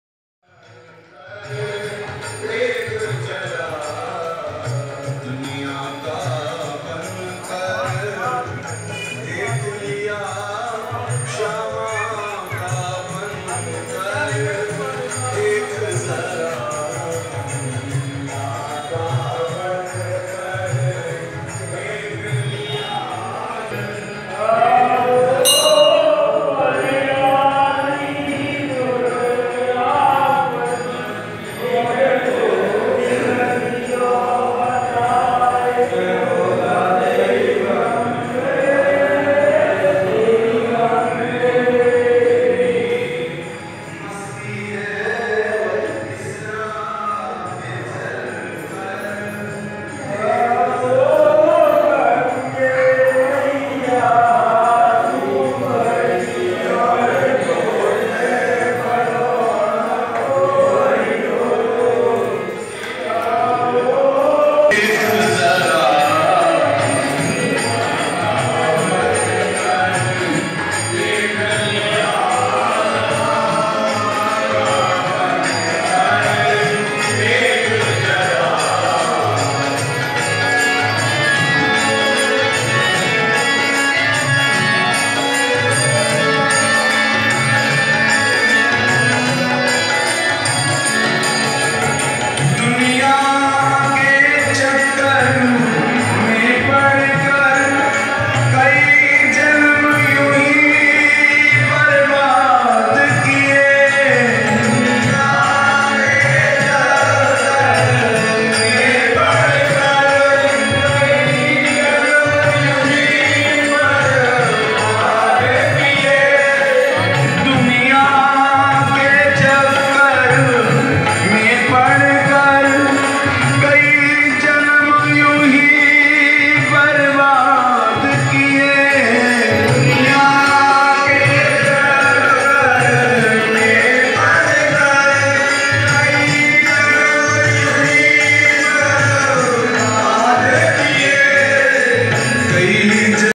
World's 2nd bankey bihari Temple...Same as vrindavan bankey bihari Temple mathura..
This Temple is 108 year old...It is very ancient and mysterious Temple.